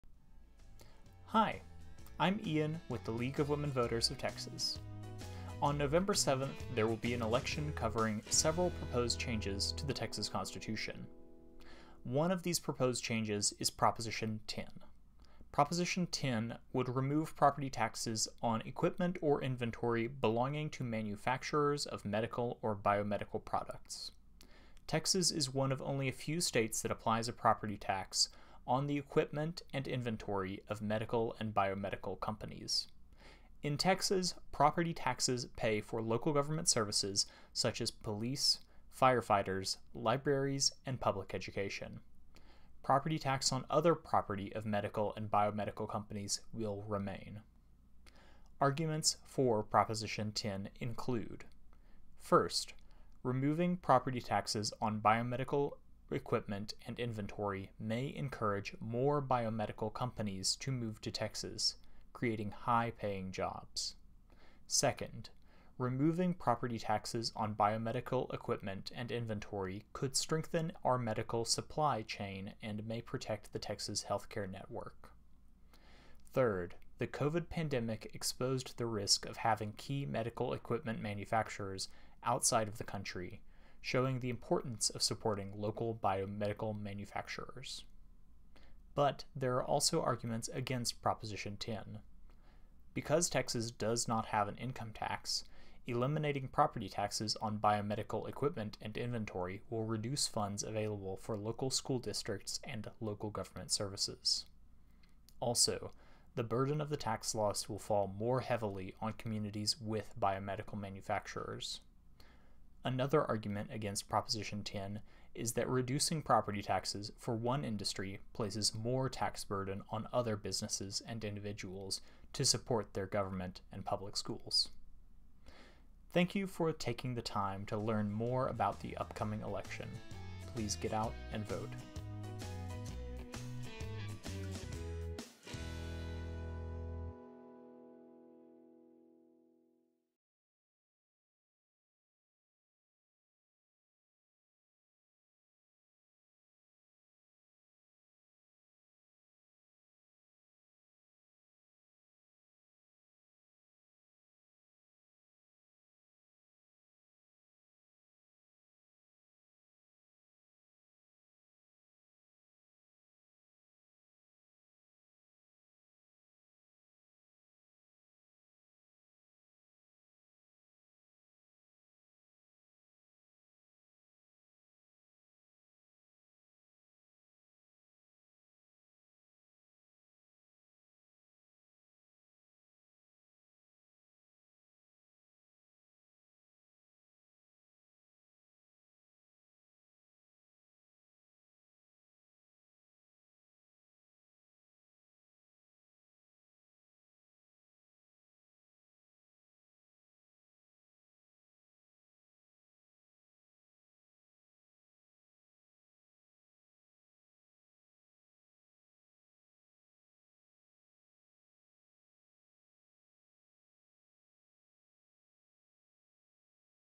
Prop 10 - November 7, 2023 Constitutional Amendment Election
On November 7th, there will be an election covering several proposed changes to the Texas Constitution.

One of these proposed changes is Proposition 10. Proposition 10 would remove property taxes on equipment or inventory belonging to manufacturers of medical or biomedical products. Texas is one of only a few states that applies a property tax on the equipment and inventory of medical and biomedical companies. In Texas, property taxes pay for local government services such as police, firefighters, libraries, and public education. Property tax on other property of medical and biomedical companies will remain.